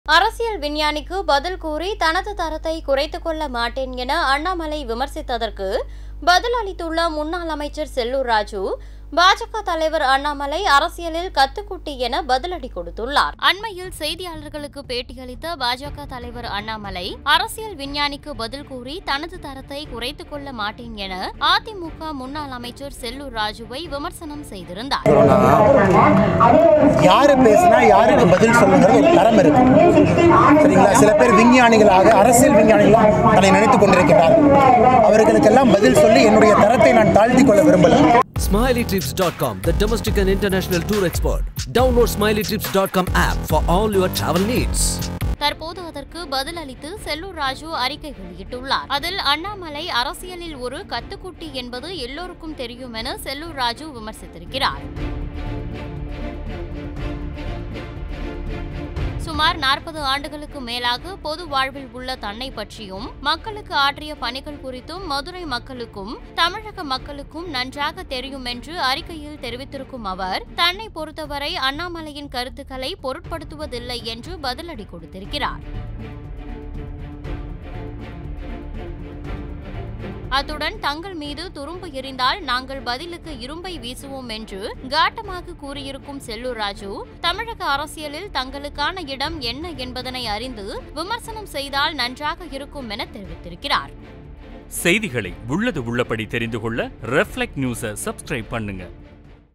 அரசியலில் அண்ணாமலை கத்துக்குட்டி என செல்லூர் ராஜு பதிலடி | Annamalai | Sellur K Raju
அரசியலில் அண்ணாமலை கத்துக்குட்டி என செல்லூர் ராஜு பதிலடி

Smiley Trips